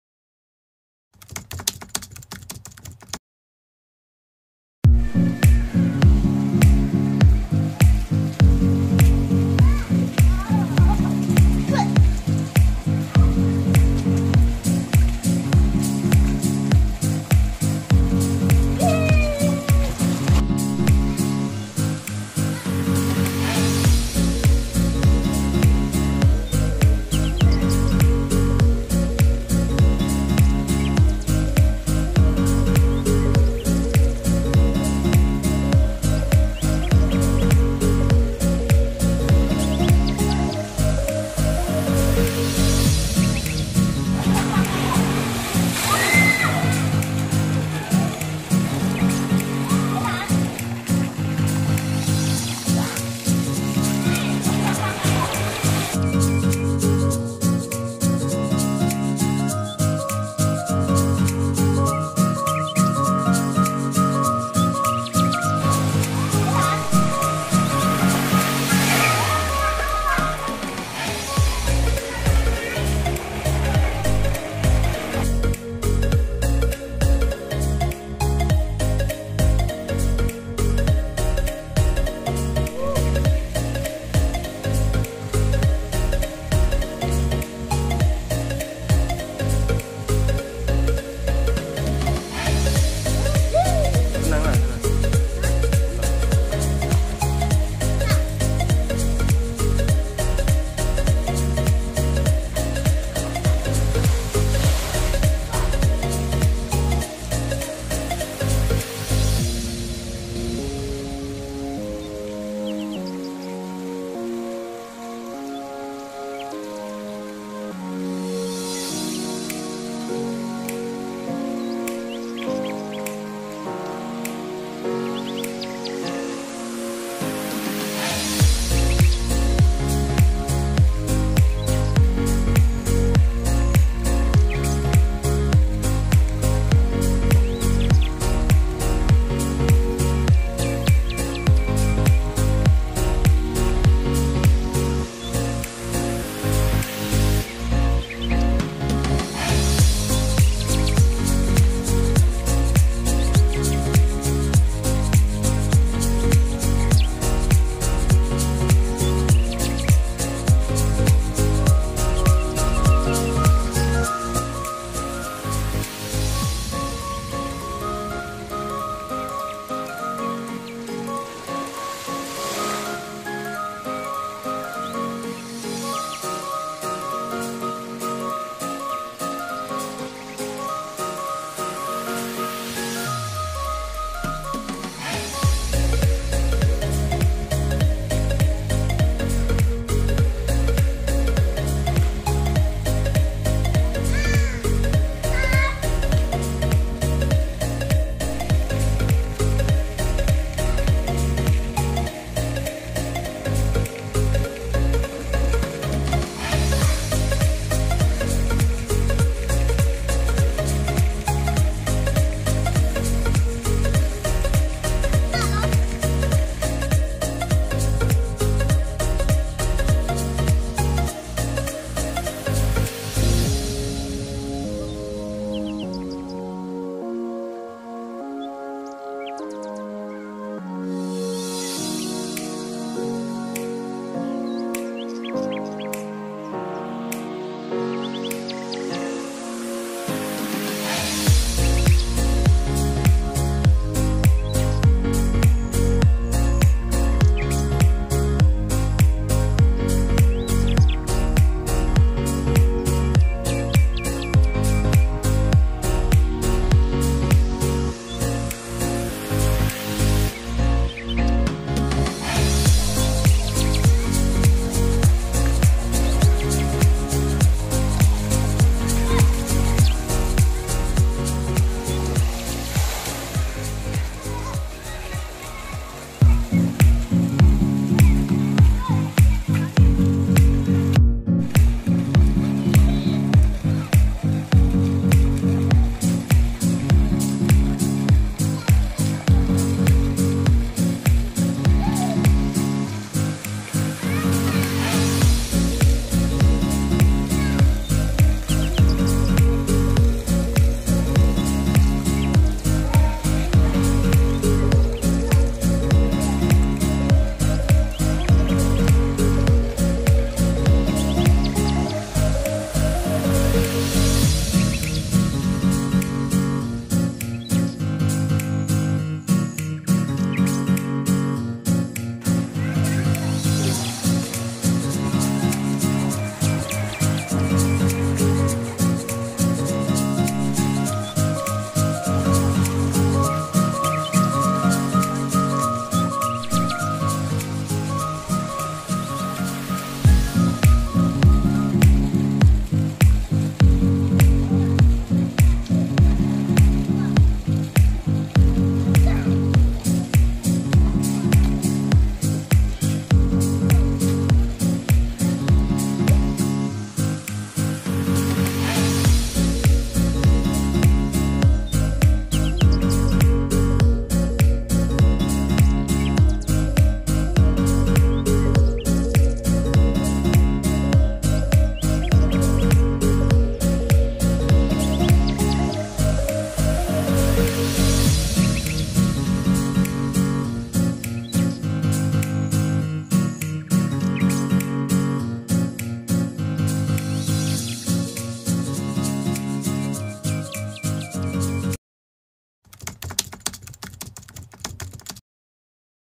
GINI Rasanya Berenang Pakai Baju Koko Swimming in the Pool السباحة فى البركة
Kolam Renang Vitra Tirta Raya Waterboom Langsa Tahun 2022